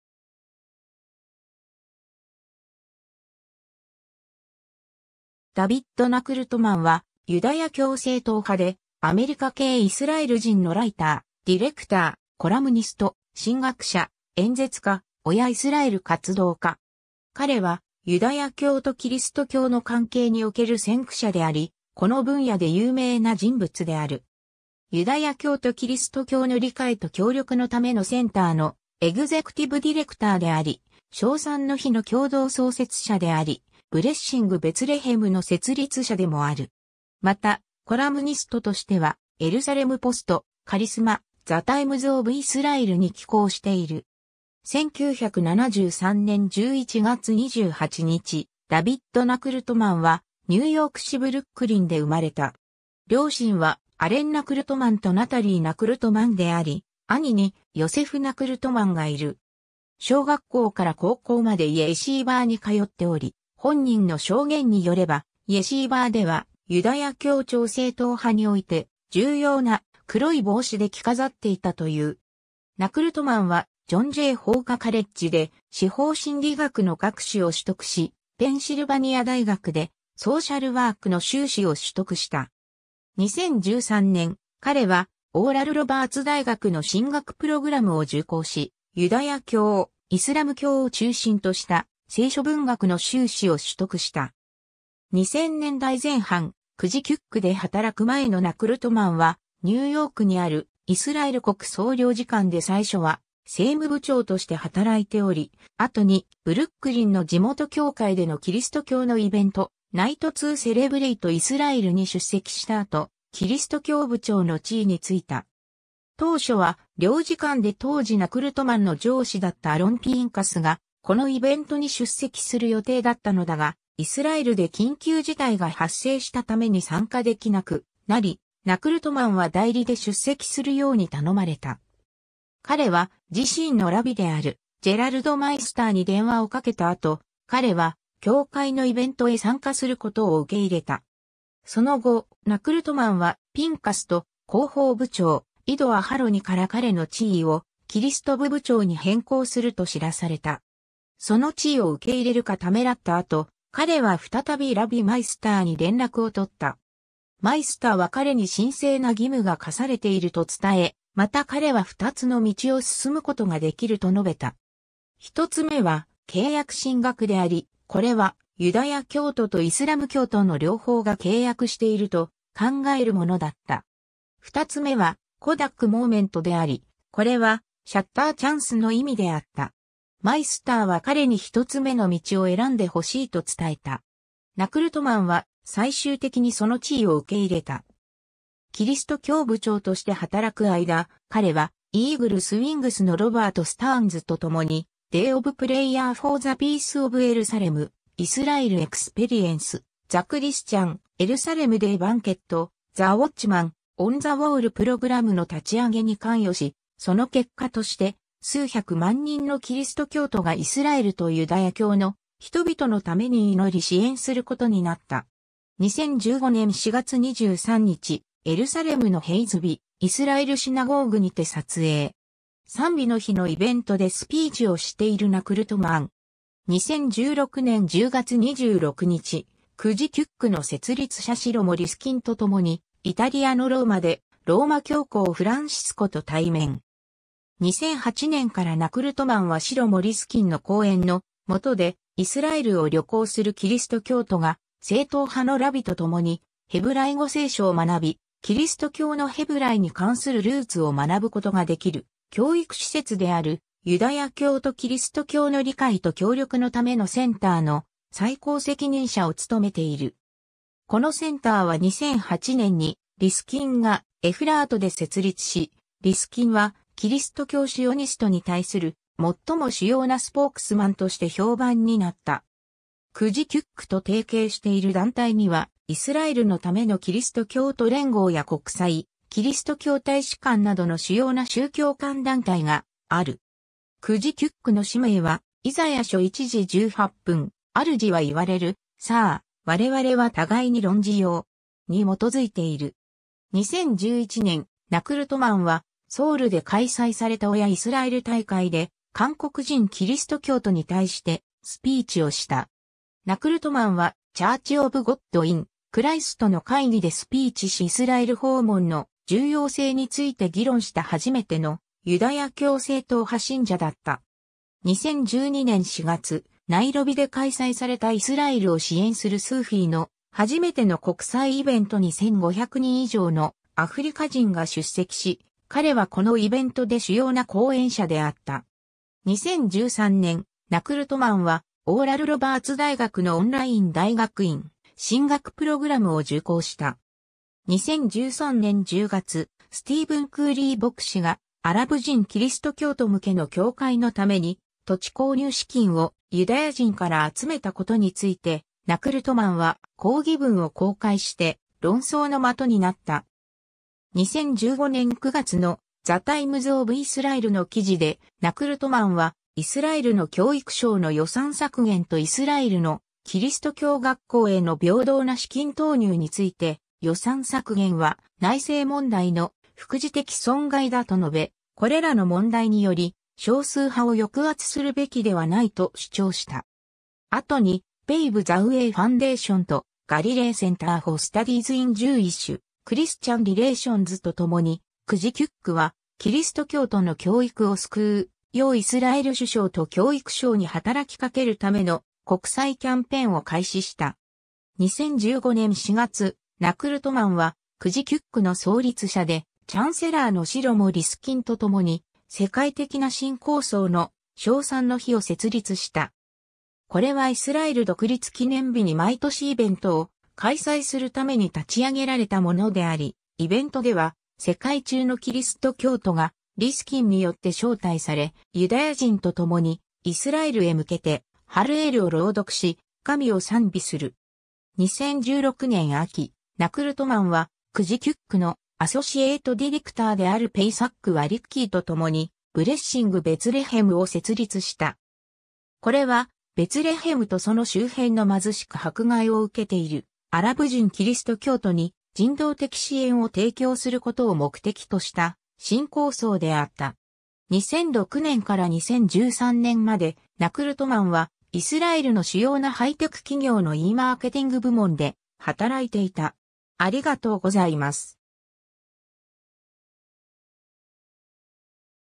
ダヴィッド・ナクルトマン
ユダヤ教・キリスト教間対話
ダヴィッド・ナクルトマン （英：David Nekrutman、ヘブライ語: דוד נקרוטמן‎、1973年11月28日 -）はユダヤ教正統派でアメリカ系イスラエル人のライター、ディレクター、コラムニスト、神学者、演説家、親イスラエル活動家。
彼はユダヤ教とキリスト教の関係における先駆者であり、この分野で有名な人物である。
『ユダヤ教とキリスト教の理解と協力のためのセンター』 (CJCUC) のエグゼクティブ・ディレクターであり、『賞賛の日』の共同創設者であり、『Blessing Bethlehem（直訳：ベツレヘムへの祝福）』の設立者でもある。
また、コラムニストとしては『エルサレム・ポスト』、『Charisma』、『The Times of Israel』に寄稿している。
 1973年11月28日、ダヴィッド・ナクルトマンはニューヨーク市ブルックリンで生まれた。
両親はアレン・ナクルトマン (Allen Nekrutman) とナタリー・ナクルトマン (Natalie Nekrutman、旧姓・ベル Bell) であり、兄にヨセフ・ナクルトマン (Joseph Nekrutman) がいる。
小学校から高校までイェシーバーに通っており、本人の証言によればイェシーバーではユダヤ教超正統派において重要な「黒い帽子」で着飾っていたという。
 ナクルトマンはジョン・ジェイ法科カレッジで司法心理学の学士を取得し、ペンシルバニア大学でソーシャルワークの修士を取得した。
2013年、彼はオーラル・ロバーツ大学の神学プログラムを受講し、ユダヤ教・イスラム教を中心とした聖書文学の修士を取得した。
 2000年代前半、CJCUCで働く前のナクルトマンはニューヨークにあるイスラエル国総領事館で最初は政務部長として働いており、後にブルックリンの地元協会でのキリスト教のイベント「Night to Celevrate Israel（直訳：イスラエルを祝う夜）」に出席した後、キリスト教部長の地位についた。
当初は領事館で当時ナクルトマンの上司だったアロン・ピンカスがこのイベントに出席する予定だったのだが、イスラエルで緊急事態が発生したために参加できなくなり、ナクルトマンは代理で出席するように頼まれた。
彼は自身のラビであるGerald Meisterに電話をかけた後、彼は教会のイベントへ参加することを受け入れた。
その後、ナクルトマンはピンカスと広報部長Ido Aharoniから彼の地位をキリスト部部長に変更すると知らされた。
その地位を受け入れるかためらった後、彼は再びラビMeisterに連絡をとった。
Meisterは彼に神聖な義務が課されていると伝え、また彼は2つの道を進むことができると述べた。
1つ目は契約神学であり、これはユダヤ教徒とイスラム教徒の両方が契約していると考えるものだった。
2つ目は「Kodak Moment」であり、これは「シャッターチャンス」の意味であった。
Meisterは彼に1つ目の道を選んでほしいと伝えた。
ナクルトマンは最終的にその地位を受け入れた。
キリスト教部長として働く間、彼はEagles' WingsのRobert Stearnsと共に『Day of Prayer for the Peace of Jerusalem（直訳：エルサレムの平和を祈る日）』、『Israel Experience』、『The Christian Jerusalem Day Banquet』、 The Watchman on the Wall programの立ち上げに関与し、その結果として数百万人のキリスト教徒がイスラエルとユダヤ教の人々のために祈り支援することになった。
 2015年4月23日、エルサレムのHaZvi Israelシナゴーグ（ユダヤ教の教会）にて撮影。
『賛美の日』のイベントでスピーチをしているナクルトマン。
 2016年10月26日、CJCUCの設立者シロモ・リスキンと共にイタリアのローマでローマ教皇フランシスコと対面。
 2008年からナクルトマンはシロモ・リスキンの後援の下で、イスラエルを旅行するキリスト教徒が正統派のラビと共にヘブライ語聖書を学び、キリスト教のヘブライに...